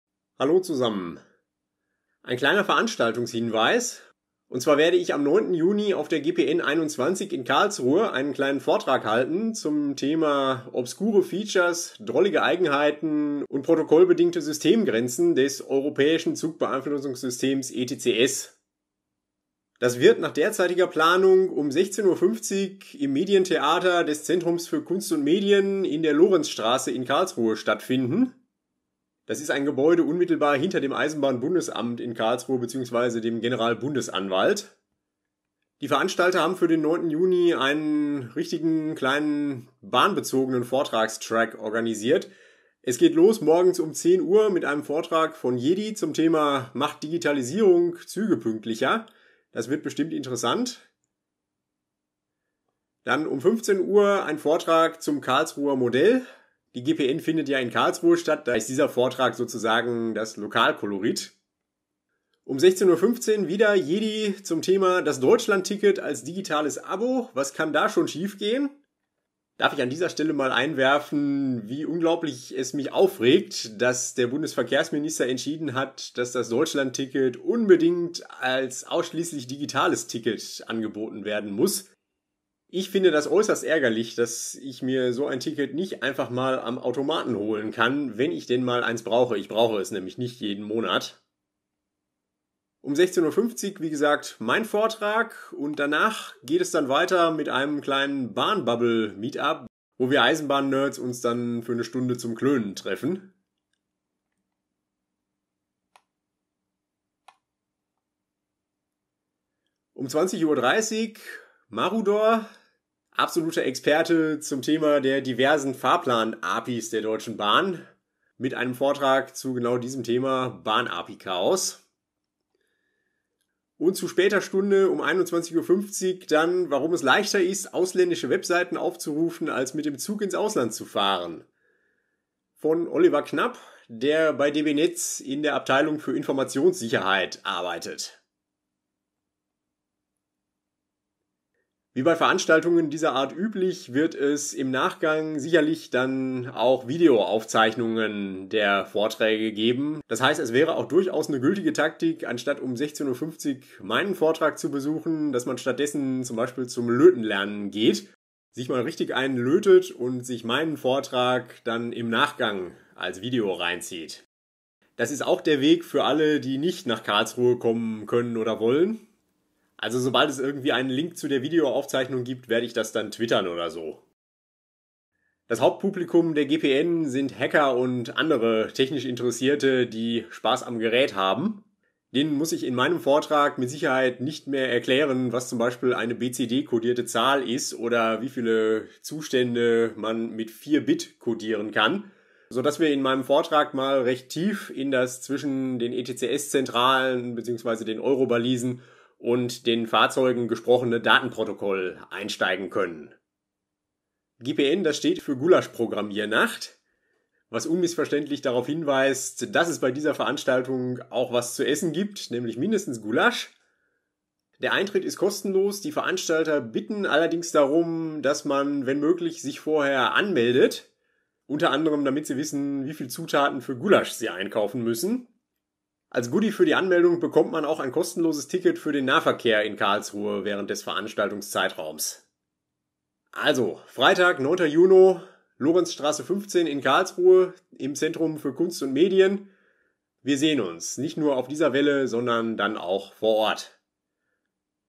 Veranstaltungshinweis: ETCS-Vortrag von mir auf der GPN21 in Karlsruhe
Am 9.6.23 gibt es von mir einen kleinen Vortrag zum Thema obskure Features, drollige Eigenheiten und Systemgrenzen des ETCS auf der Gulaschprogrammiernacht in Karlsruhe